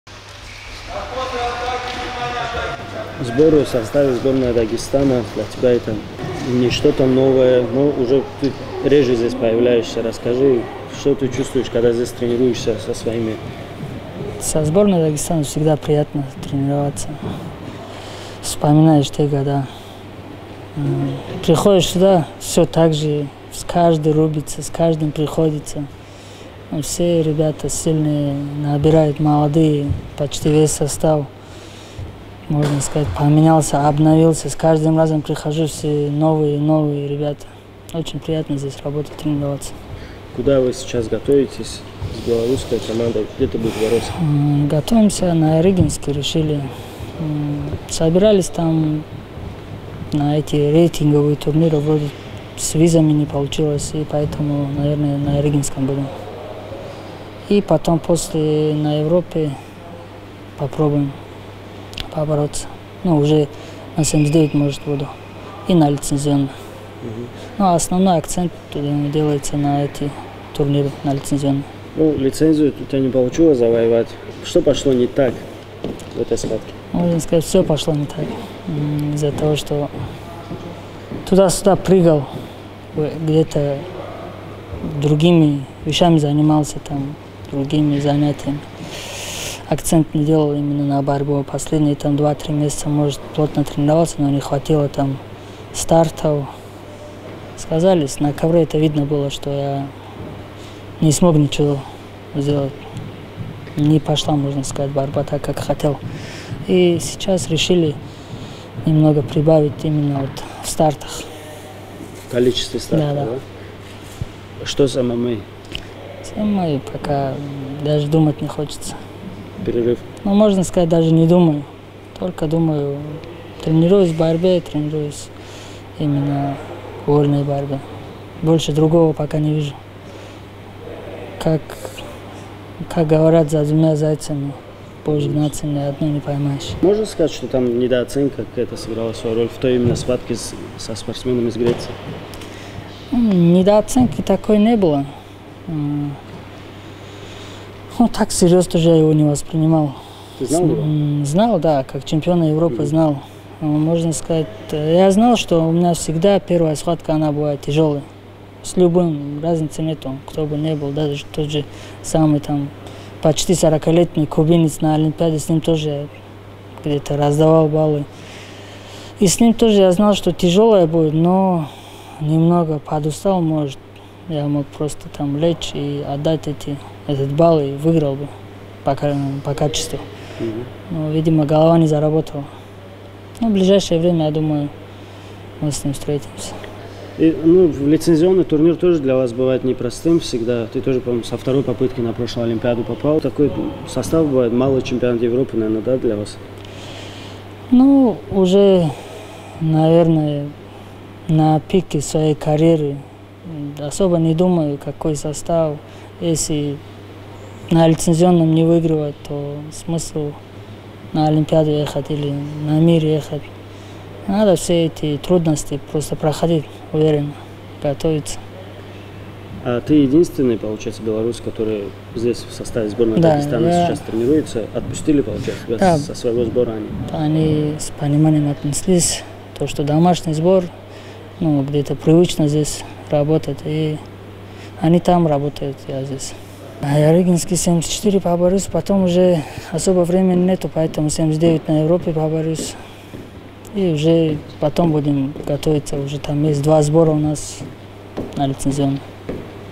Магомедхабиб Кадимагомедов: "Про ММА пока не думаю"
Серебряный призёр Олимпиады в Токио Магомедхабиб Кадимагомедов, готовящийся к новому сезону вместе с дагестанскими вольниками ответил на вопросы корреспондента wrestdag.ru. Дагестанский легионер сборной Беларуси рассказал Курбану Рагимову о тренировках со своими земляками, поведал о ближайших планах и о некоторых изменениях в своём тренировочном процессе после неудачи на лицензионном чемпионате мира в Белграде.